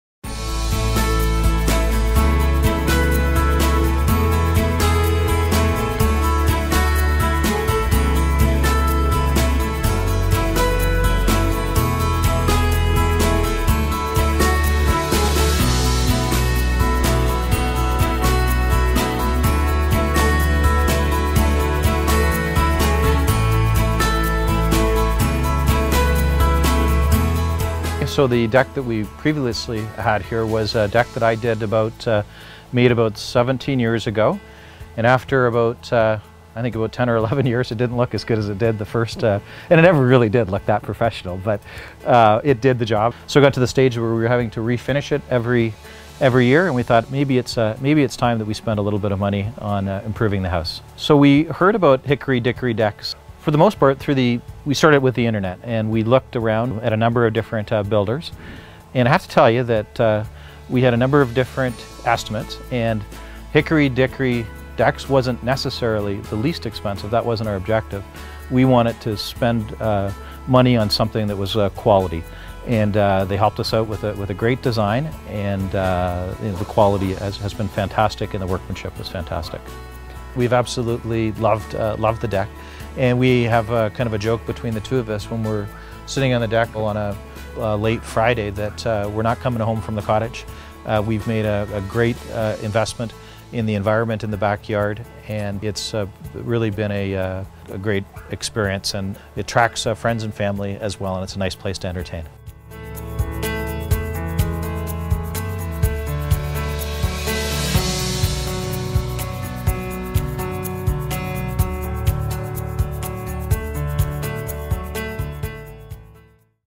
Cottage Living...At Home! Hickory Dickory Decks Hamilton
This incredible TREX deck was made for long summer days by the pool and great memories.
Brad Arnold, owner of HickoryDickoryDecks Hamilton, is ready to help you enjoy life outdoors!